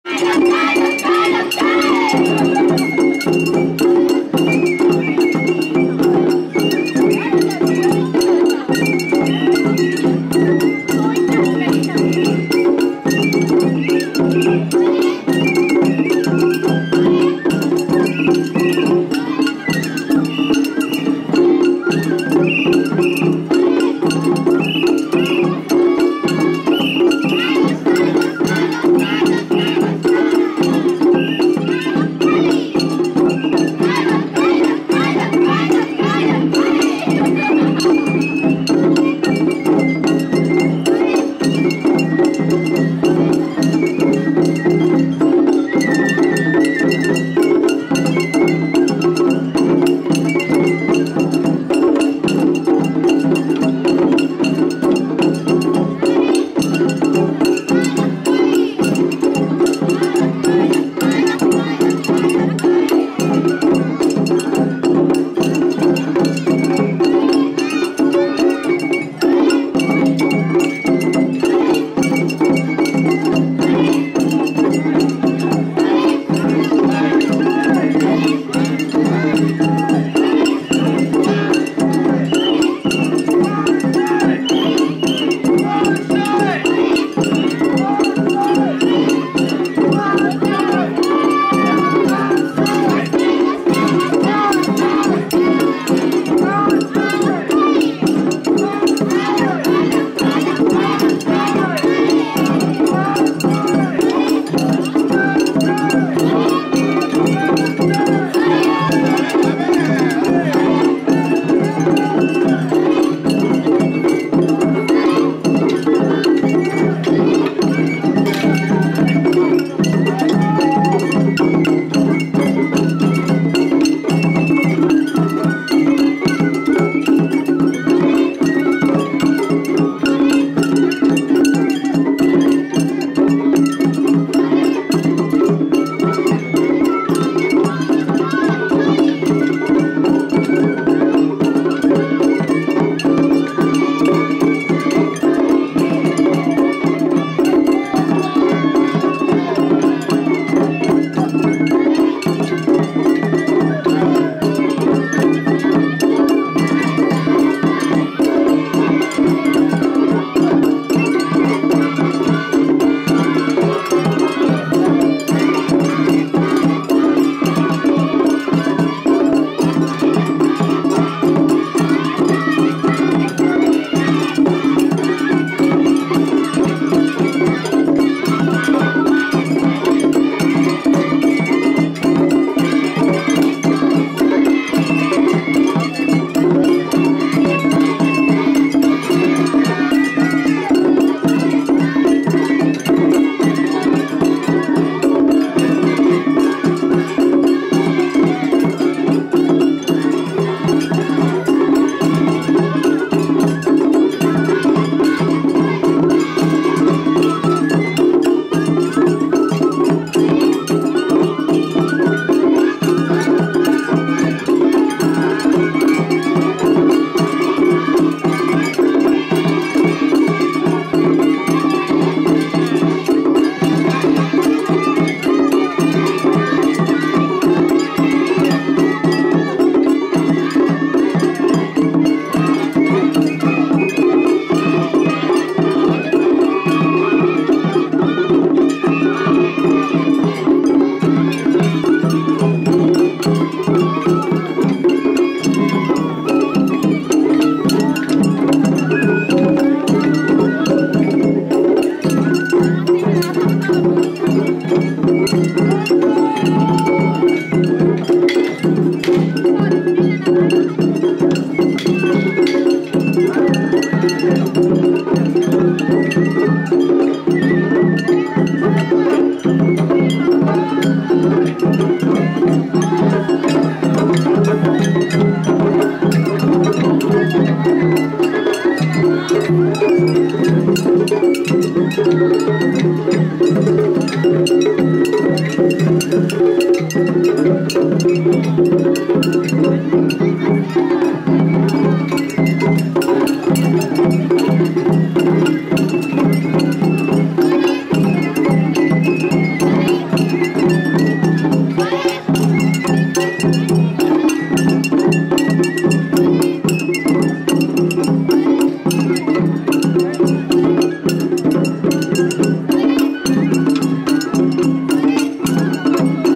ところざわまつり（所沢祭り）２０１４ 所沢商和会屋台・御幸町山車 曳っかわせ 2014.10.12 Tokorozawa Matsuri
ところざわまつり（所沢祭り）２０１４　所沢商和会屋台・御幸町山車　曳っかわせ